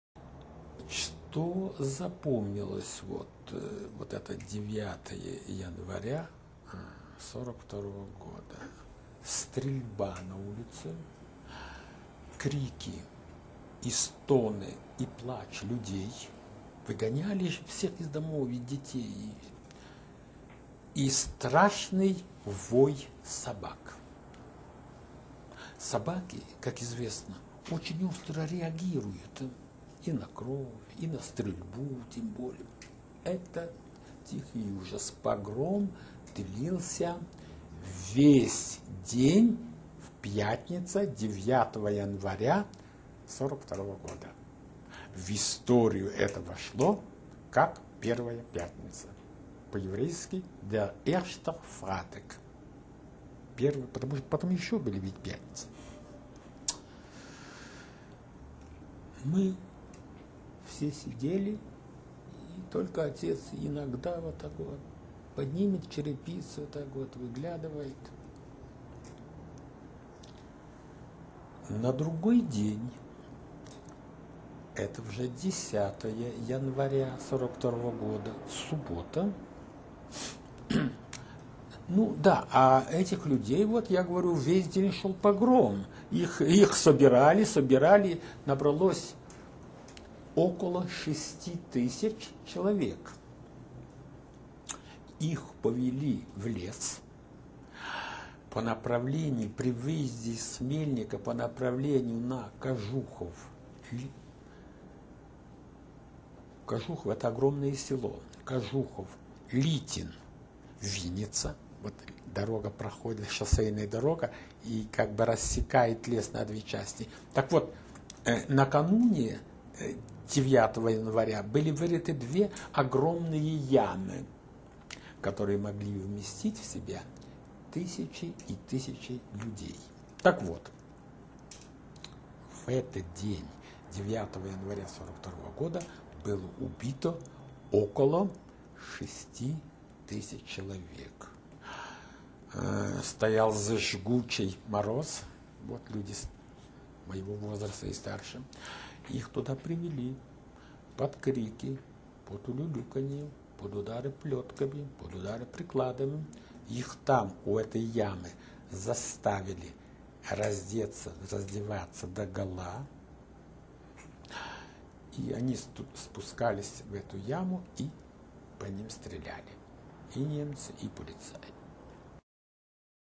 Testimony of Igor Barinshtein about the mass murder of the Jews from Khmelnik in Ukraine
Igor Barinshtein, who was born in 1932 in Khmelnik, Ukraine, testifies about the mass murder of Jews from Khmelnik. The victims were shot and buried in ditches in Ugrinovka.